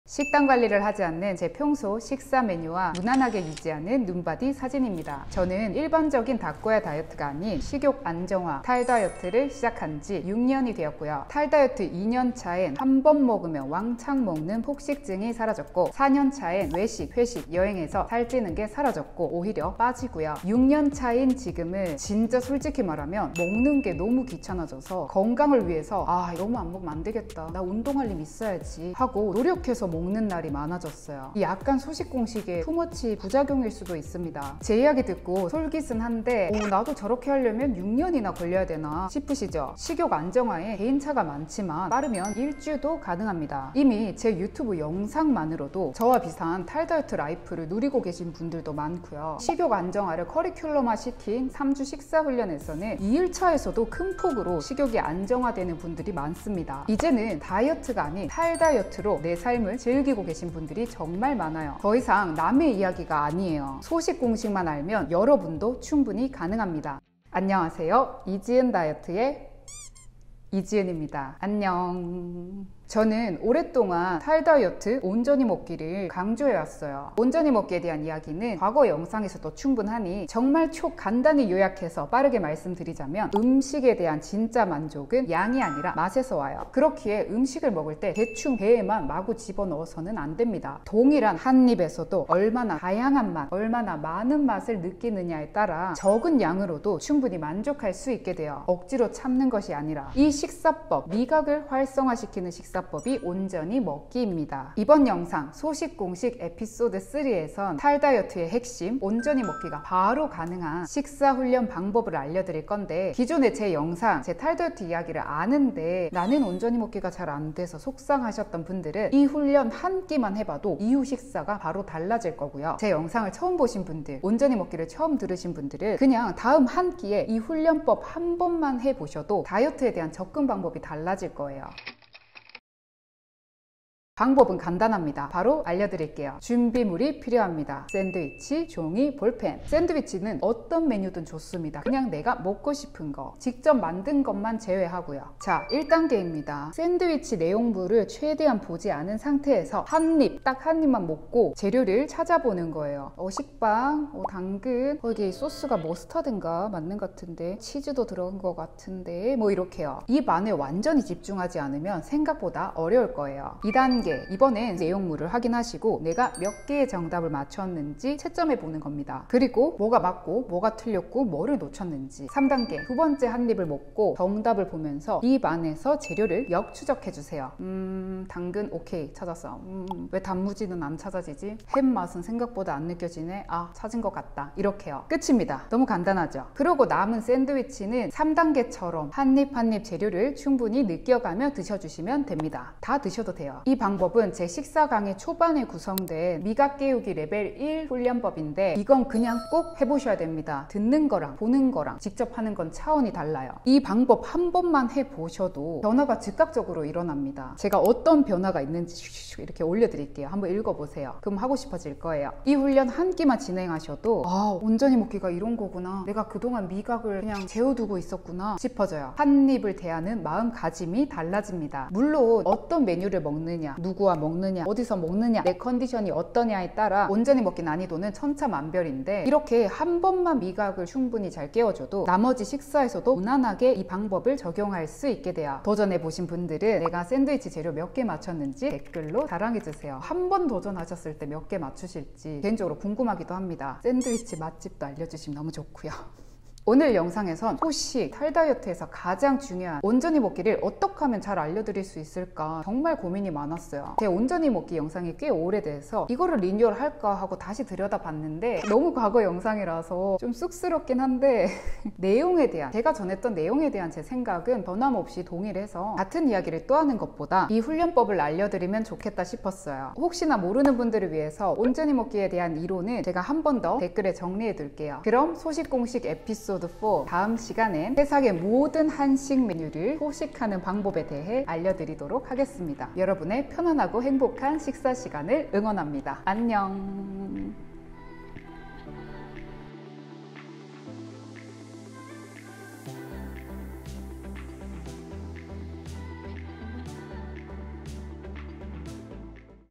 딱 '한끼'만 적용해보세요. 효과가 바로 나타납니다.
Ep.3 가장 빠르게 소식좌 되는 식사법
[소식공식 에피소드 1-8]

🔹살이 쉽게 빠지는 라이프스타일 만들기 '비가나 21days'

다이어트는 건강한 습관을 길들이는 과정입니다.

Chapter
00:00~01:08 인트로
01:08~04:34 소식공식
04:34~ 아웃트로